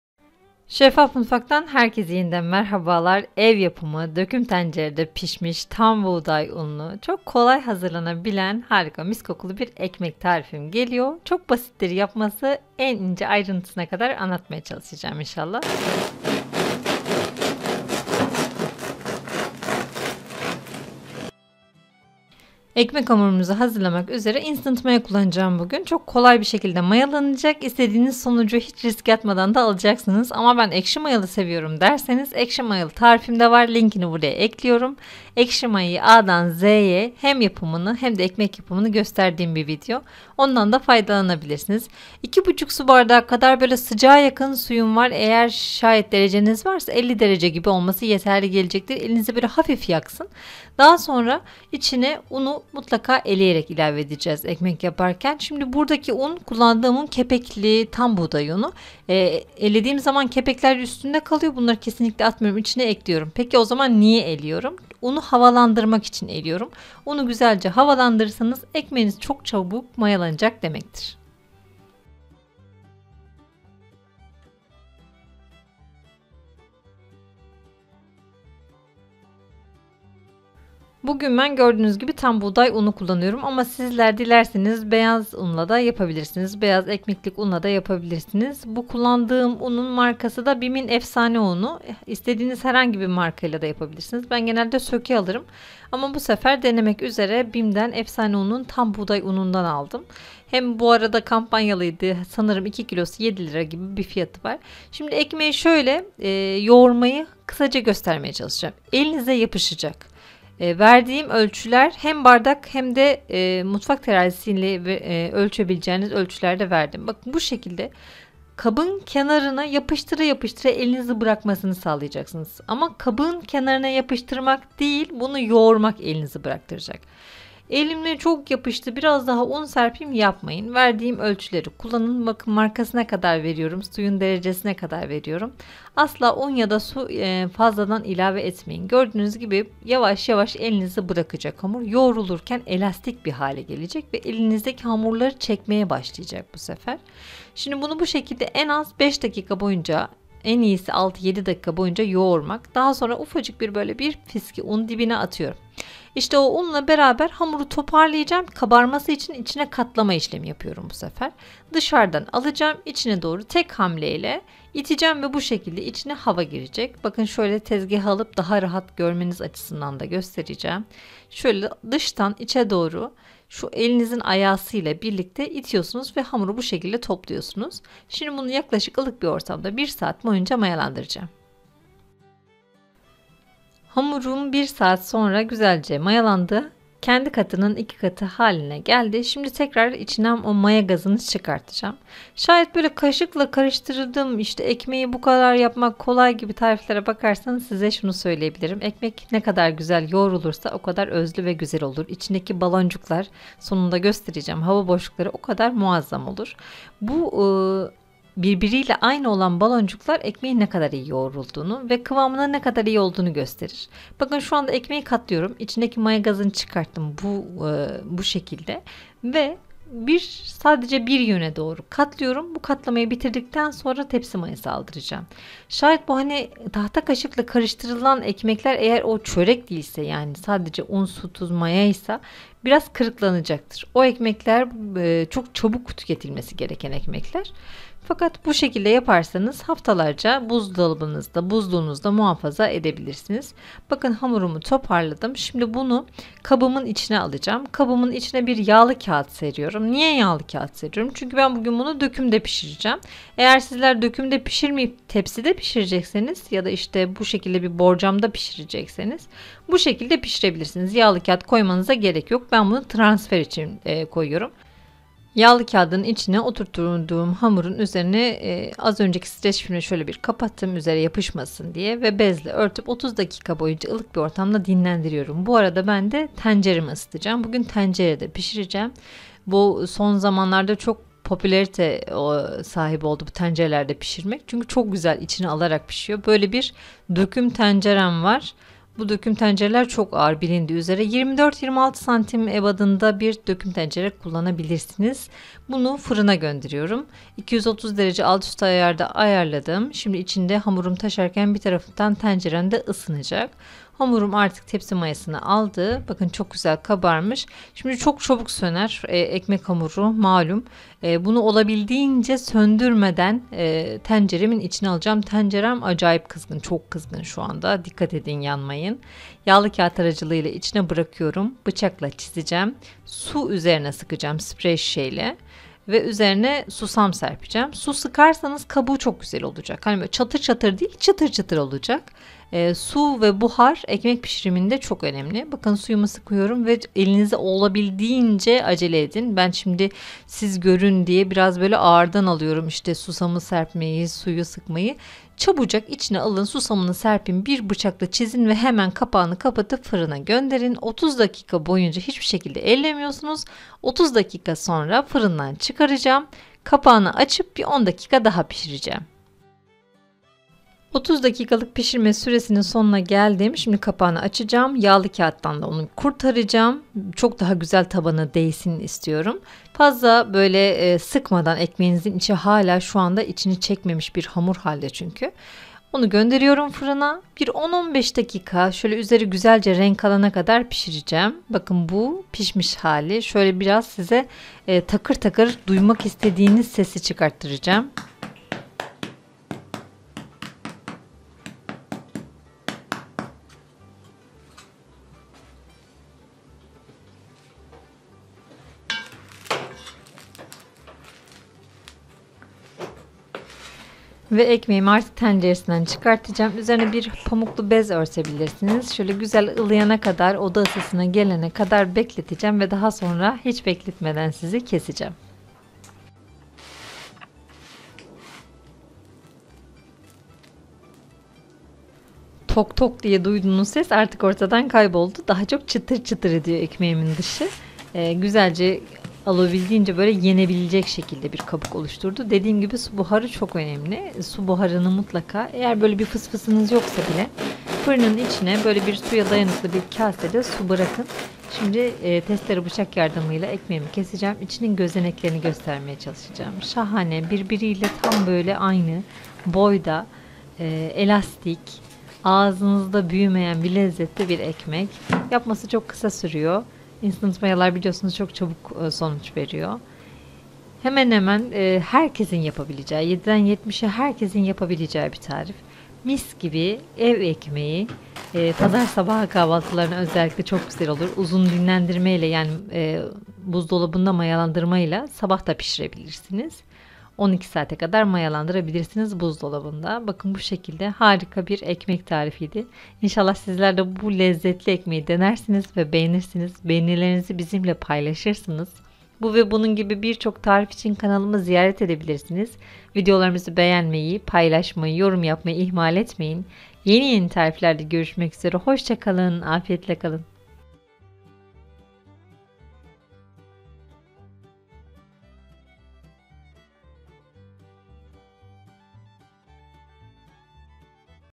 EKMEĞİNİ ELLETME 🍞EVDE KOLAYCA HAZIRLA İNSTANT MAYA İLE UZUN UZUN BEKLEMEDEN👌🏻
🍞MİS GİBİ EKMEK KOKULARI SARSIN EVİNİZİ😍😍
💁🏼EVDE YAPABİLECEĞİNİZ TAM TUTAN BİR EKMEK TARİFİ 👌🏻👌🏻
🍞HEM TAM BUĞDAY UNU HEMDE BEYAZ UNLA YAPABİLİRSİNİZ💯

🍞MALZEMELER🍞

SU BARDAĞI ÖLÇÜM 200 ML DİR
 1 PAKET İNSTANT MAYA 10 GR
2 ÇAY KAŞIĞI TUZ 15 GR
2,5 SU BARDAĞI ILIK SU
5,5 SU BARDAĞI TAM BUĞDAY UNU 600 GR

230 DERECE ÖNCEDEN ISITILMIŞ FIRINDA 30 DK KAPAKLI 10 DK KAPAKSIZ PİŞİRİYORUZ
ŞAYET DÖKÜM TENCEREDE PİŞİRMEK İSTEMEZSENİZ NORMAL TEPSİDE MAYALANDIRDIKTAN SONRA YİNE AYNI DERECEDE PİŞİREBİLİRSİNİZ

EKŞİ MAYA İLE YAPMAK İSTERSENİZ ADAN Z YE MAYANIN YAPIMI İLE BİRLİKTE EKŞİ MAYA EKMEĞİ VİDEOM İÇİN BURAYA TIKLAYIN